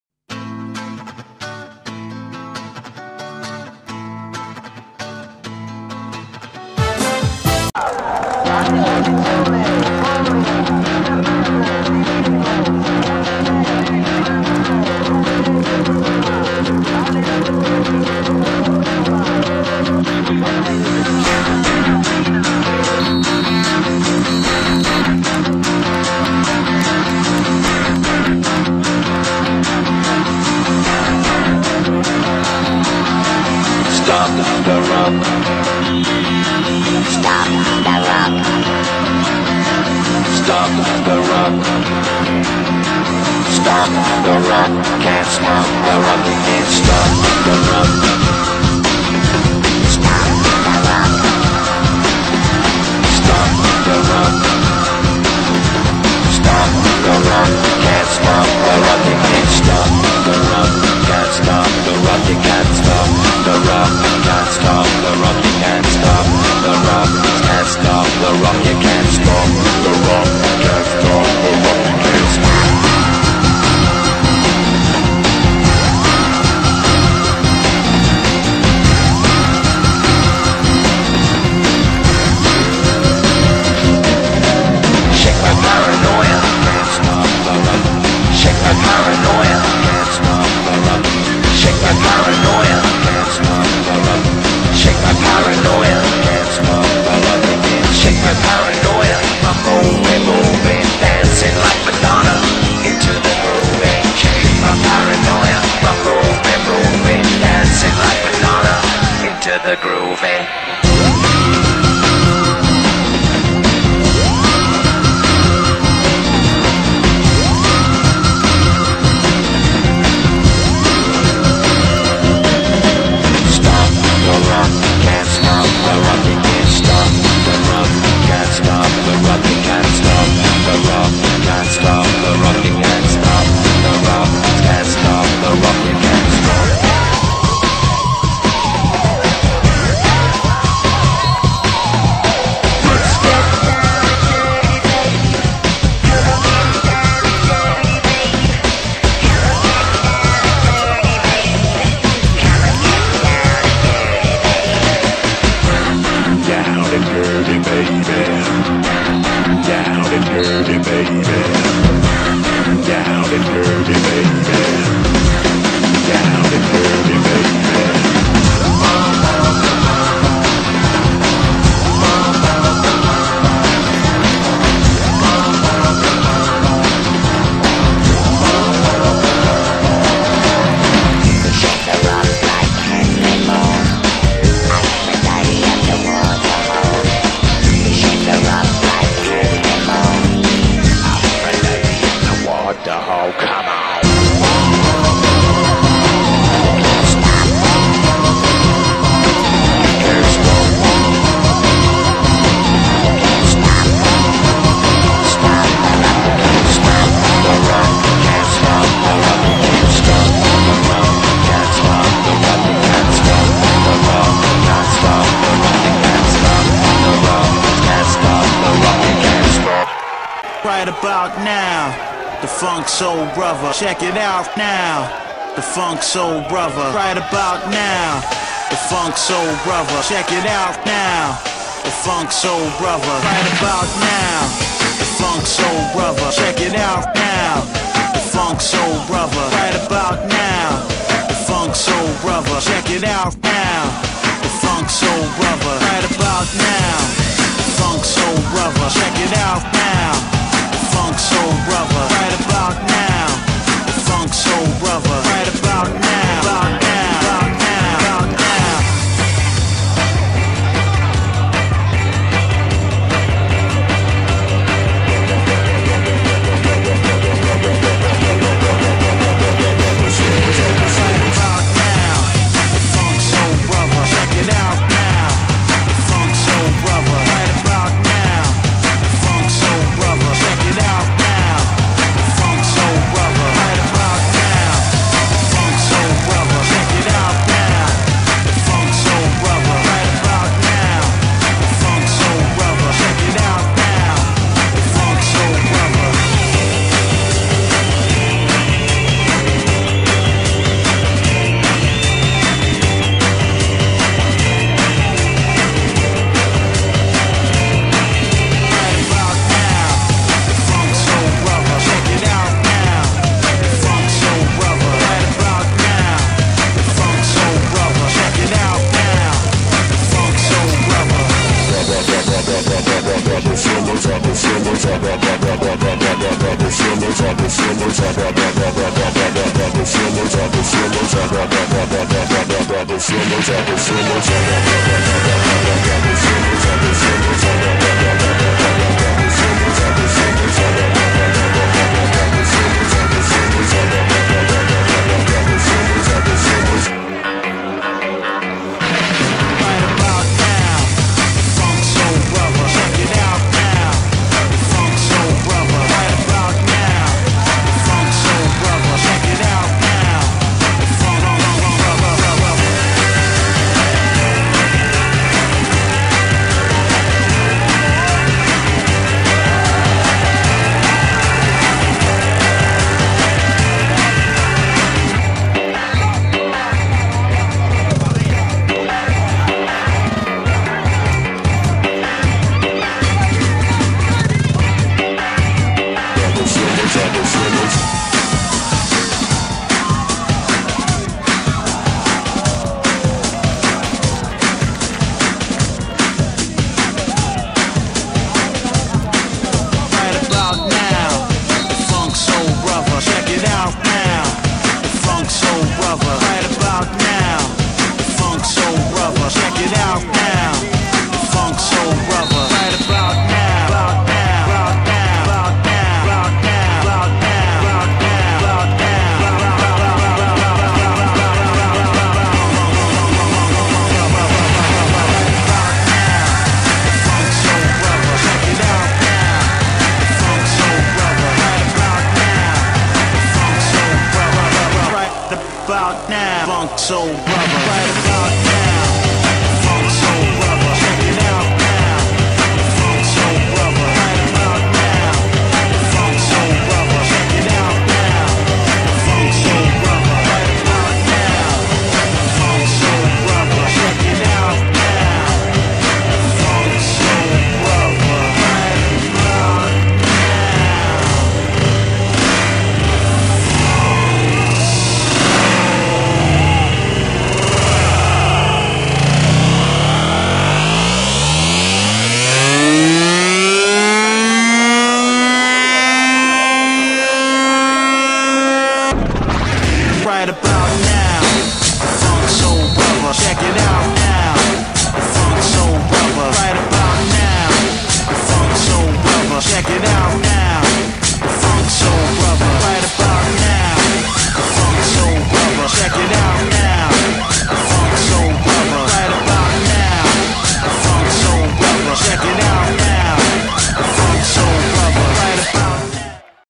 Semif. Play-off Gladiator - Foggia 2-4 (sintesi)
la sintesi della gara completa di tempi supplementari